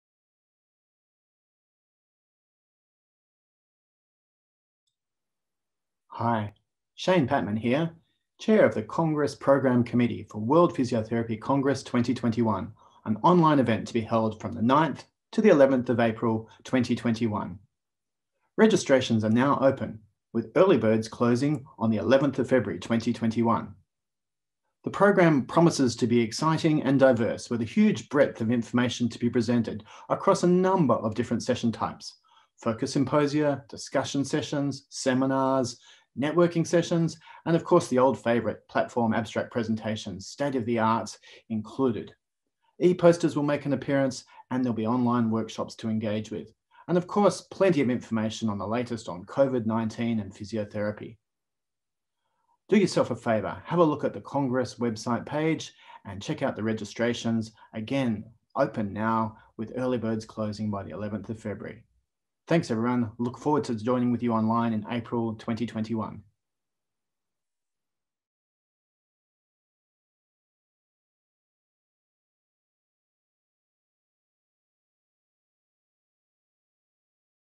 Shane Patman announces programme updates as registration opens
Congress programme committee chair, Shane Patman, highlights programme updates for World Physiotherapy Congress 2021 online as registration opens.